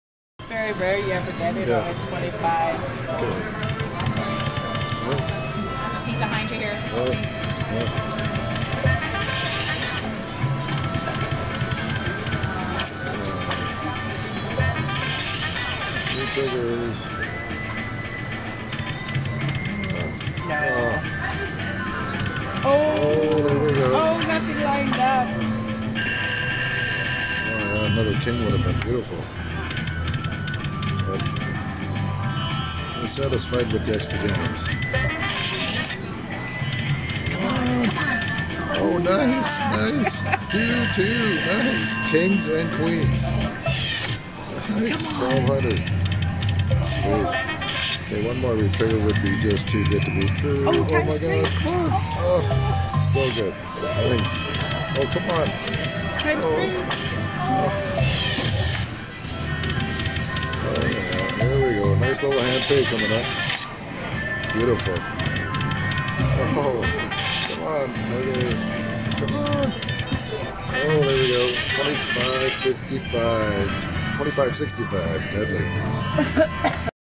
$2565 Enforcer slot free games feature on max bet gives another hand pay
Playing max bet on the Enforcer slot machine by Ainsworth and hitting the free games feature for $2565.